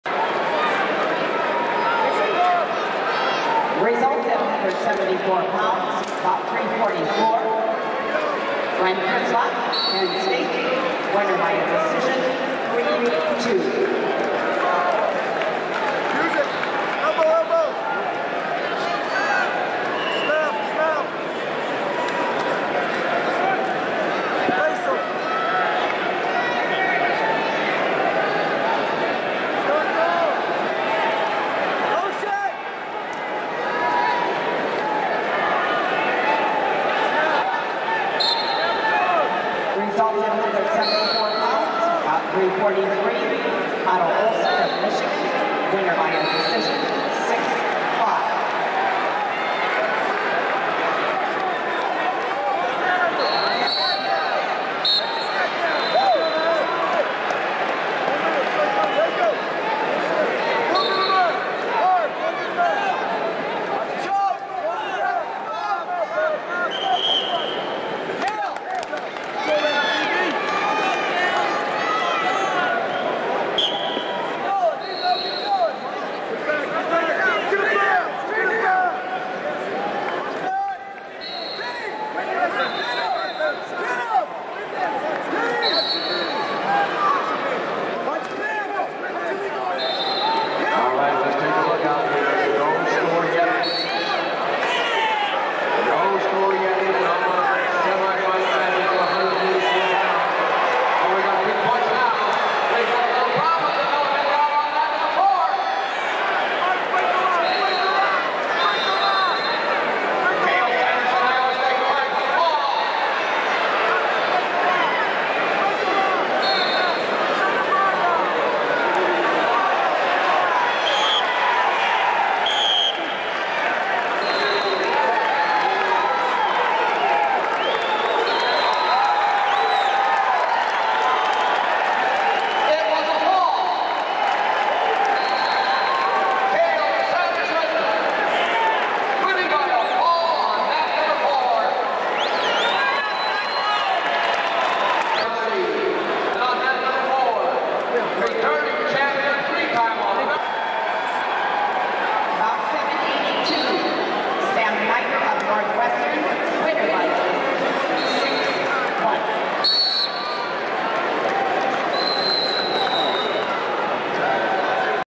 D1CW Video Vault - 1999 NCAA Semifinals - Cael Sanderson (Iowa State) vs Brad Vering (Nebraska)
Freshman Cael Sanderson is on the way to his first of four NCAA titles. He meets his eventual 2004 Olympic teammate Brad Vering in the semifinals. Both would also win NCAA titles the following year.